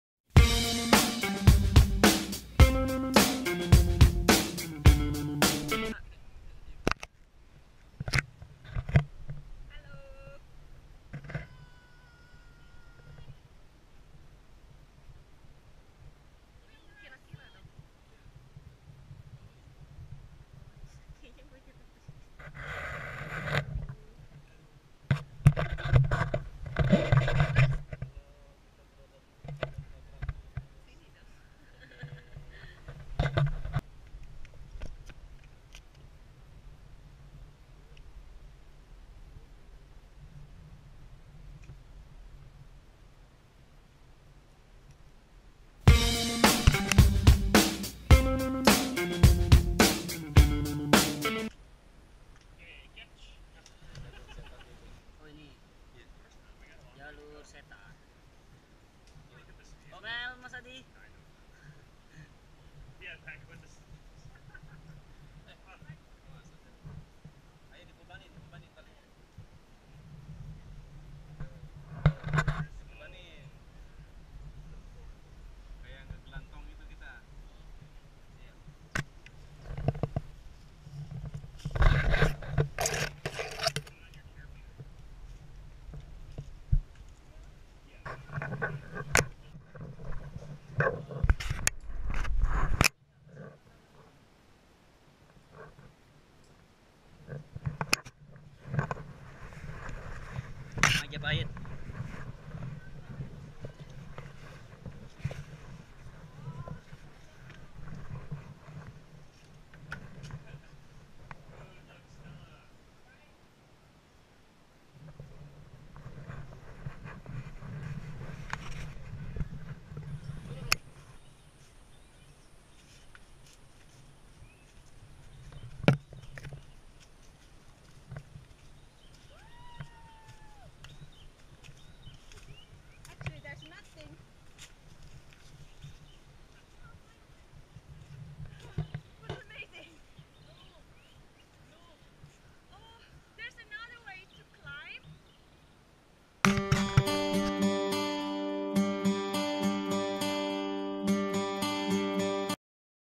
Badega Gunung Parang Via Ferrata
Via Ferrata adalah salah satu kegiatan luar ruang ruangan yang sangat menantang tetapi layak di coba. Bagi yang suka sunrise bisa naik di pagi hari di saat masih gelap, jika cuaca bagus akan terlihat semburat merah tetapi jika cuaca jelek akan tertutup oleh kabut. Jangan takut dengan kemananan alatnya karena ada pemandu yang akan menemani.